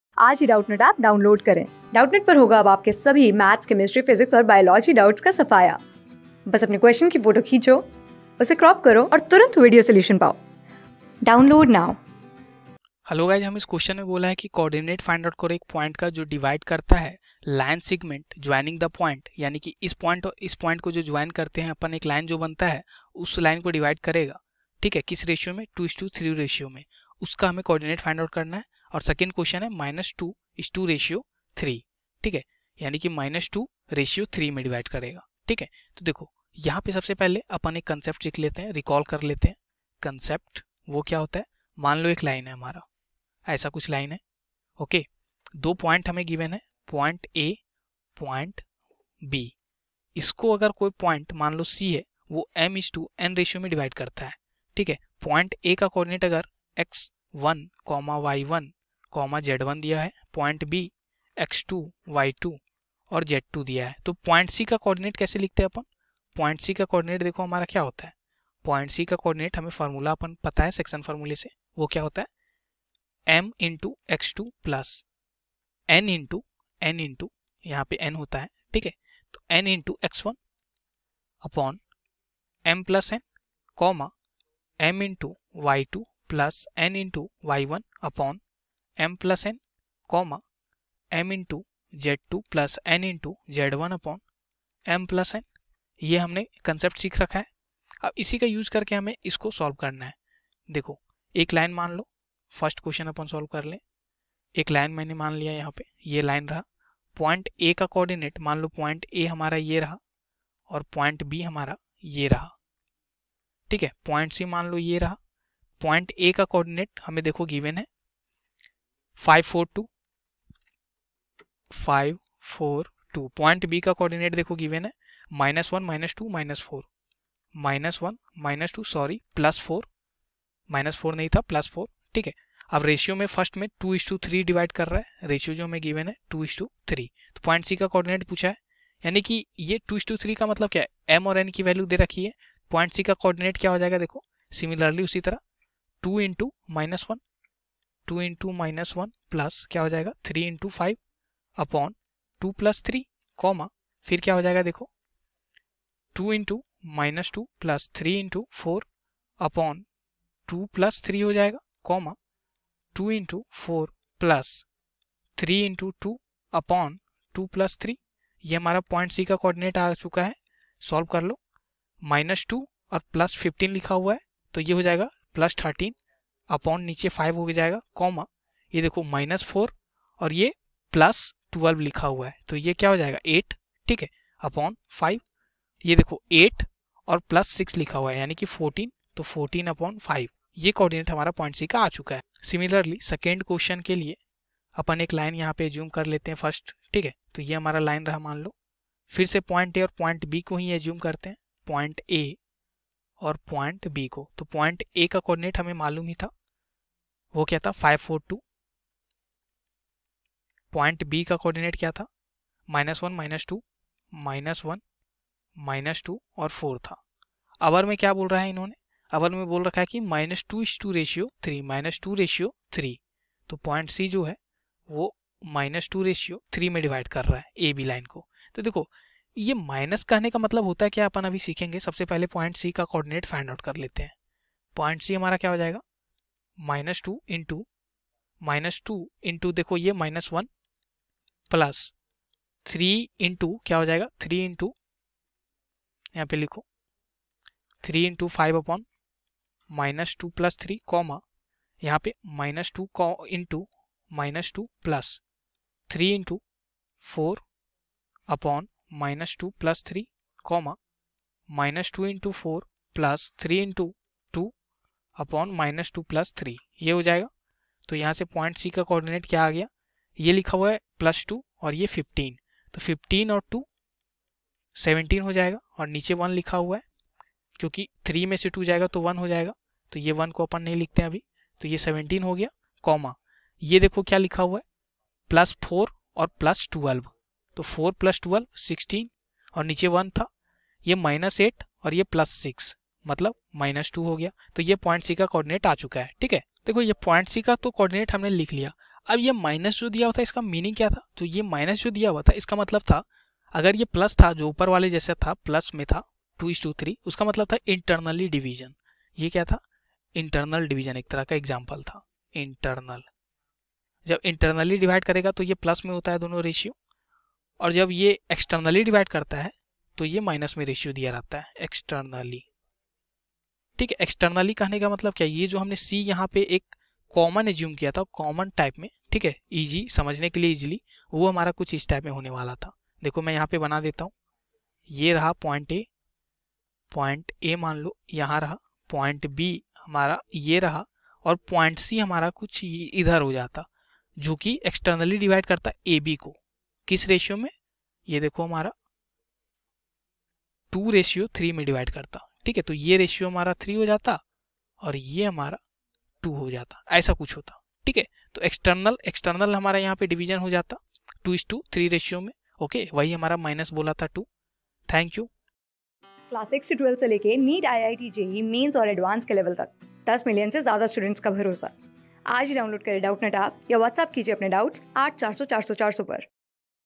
Findthe co -ordinates of the point which divides theline segment joiningthe points(5,4,2) and (-...
Findthe co -ordinates of the point which divides theline segment joiningthe points(5,4,2) and (-1,-2,4) in the ratio (i) 2:3 (ii) -2:3
Class: 11
Subject: MATHS
Chapter: INTRODUCTION TO THREE DIMENSIONAL GEOMETRY
Board:CBSE

👉 Have Any Query? Ask Us.